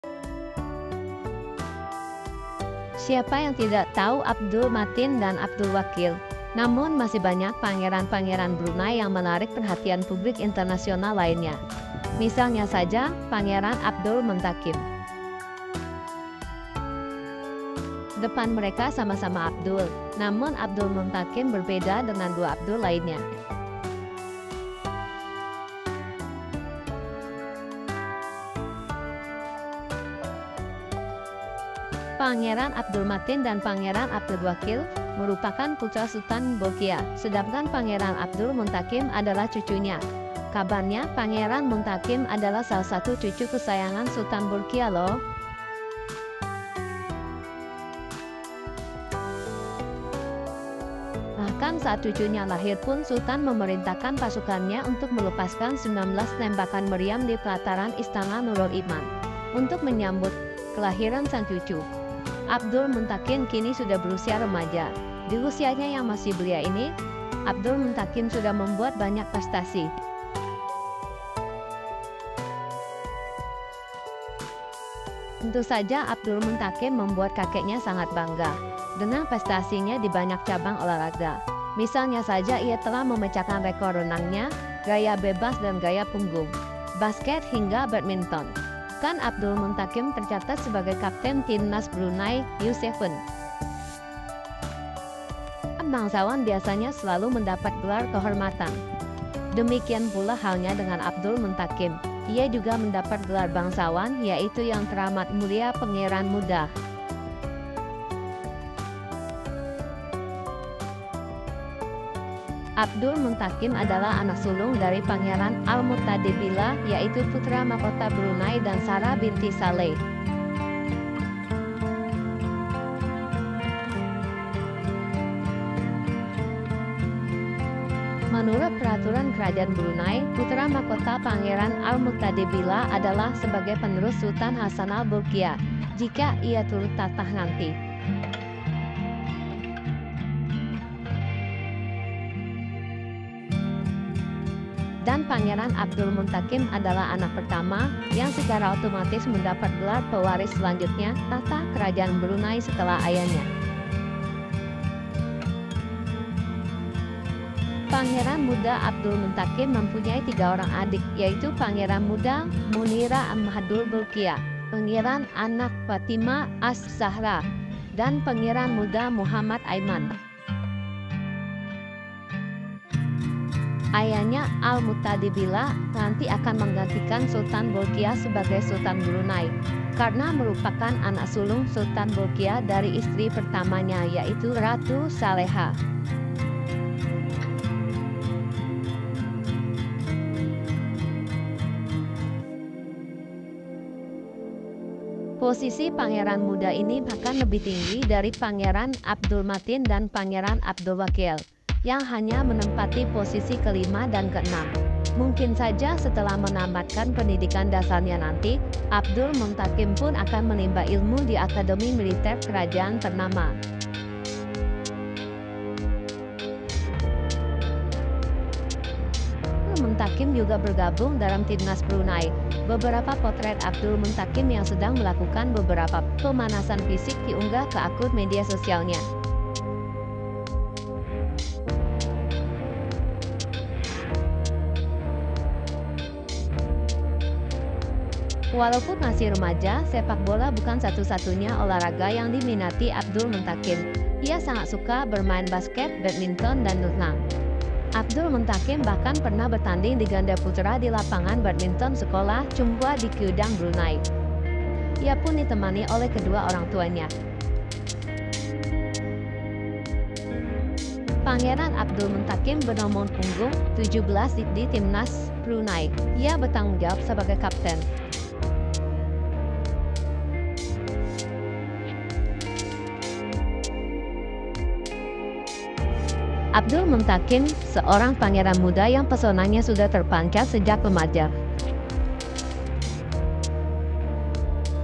Prince Abdul Muntaqim Of Brunei
Prince Abdul Muntaqim is the grandson of Sultan Hassanal Bolkiah. It is said that Abdul Muntaqim is one of her favorite grandchildren, you know. Even though he is still young, Abdul Muntaqim has made many achievements in the field of sports.